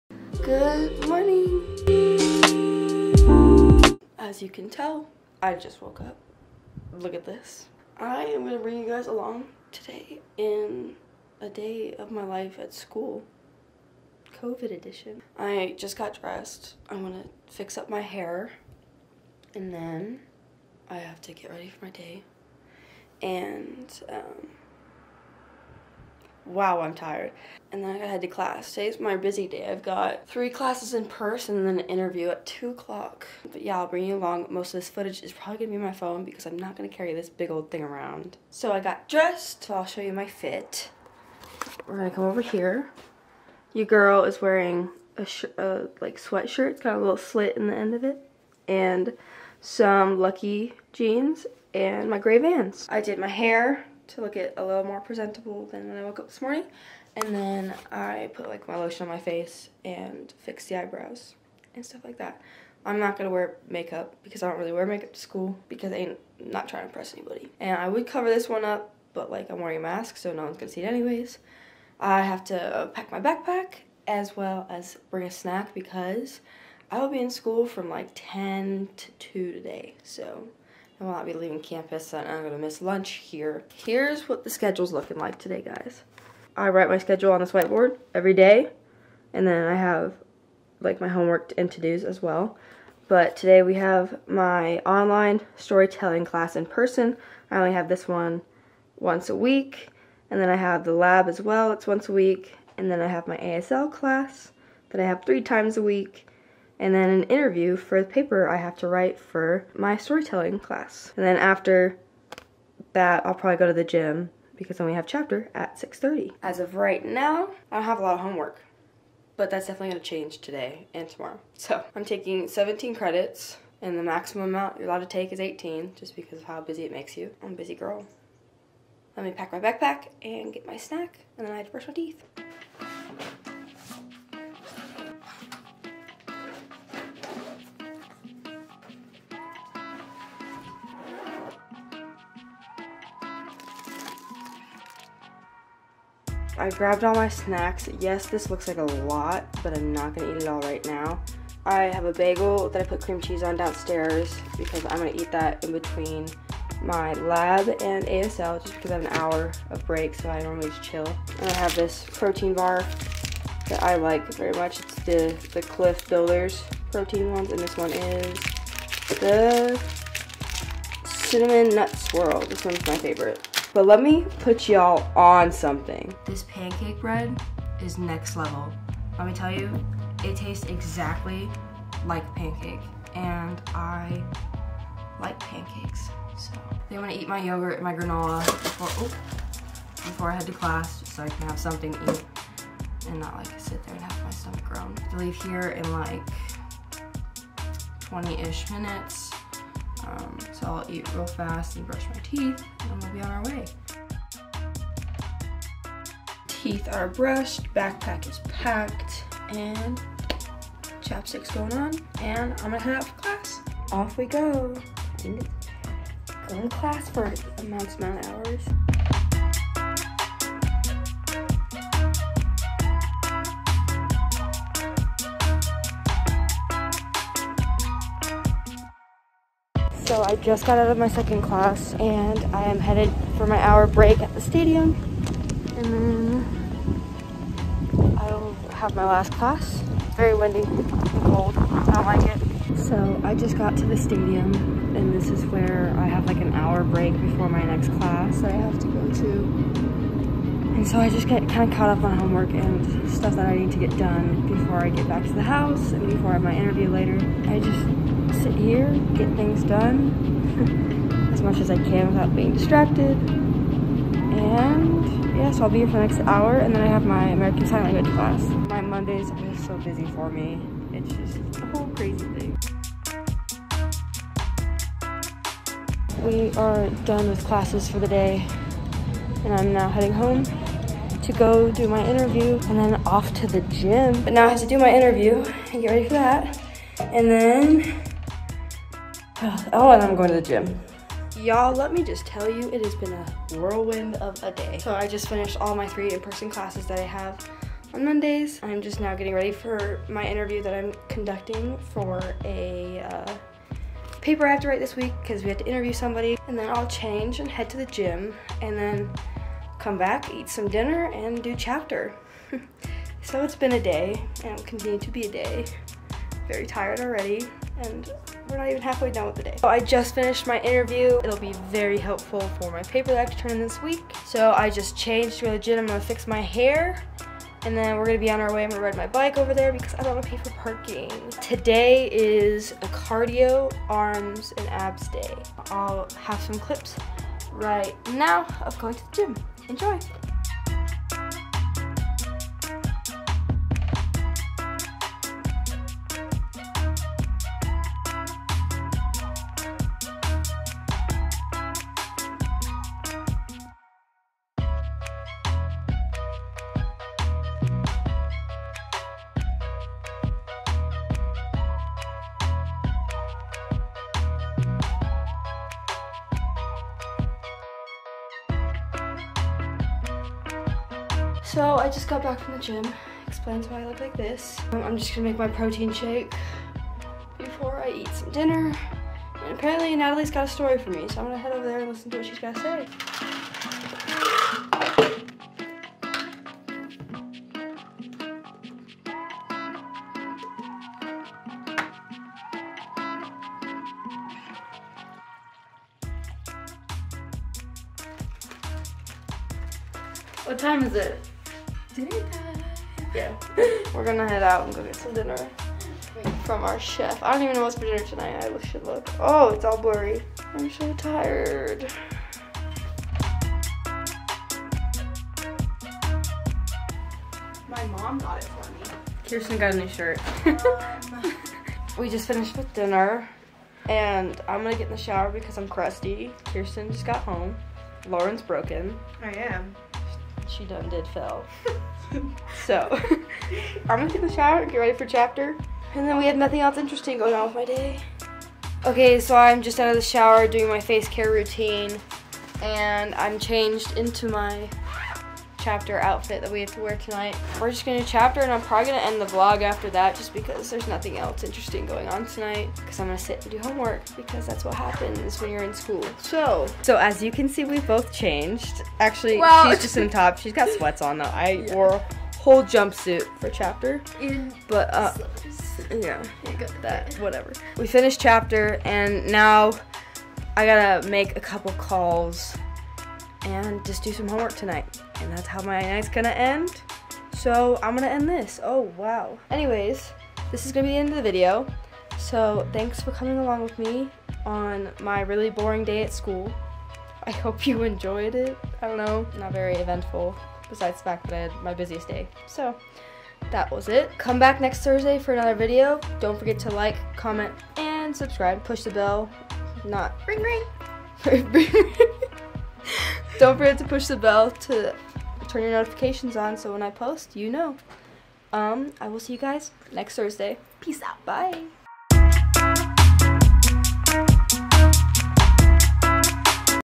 A DAY IN THE LIFE AT COLLEGE | COVID EDITION
i recorded a whole day of my life at school and what everything looks like in the new 'normal'. i also got some weird looks by doing this on campus so you're welcome 💁🏻‍♀️

VSCO: addiecavo